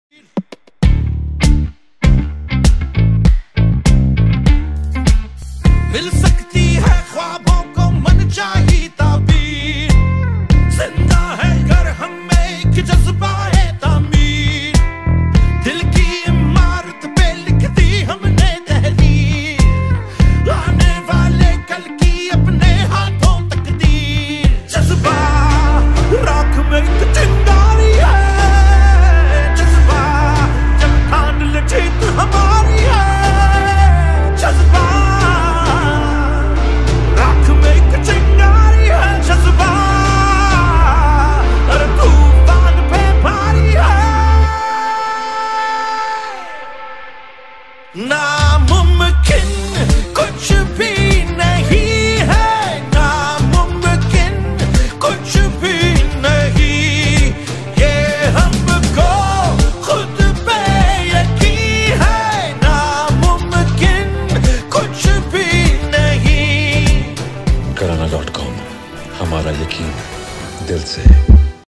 Projection Mapping at Miner-e-Pakistan
We created the tallest 3D Projection Mapping of Pakistan on the facade of Minar-e-Pakistan. It was part of Graana.com's nationwide launch campaign HamaraYaqeenDilSe.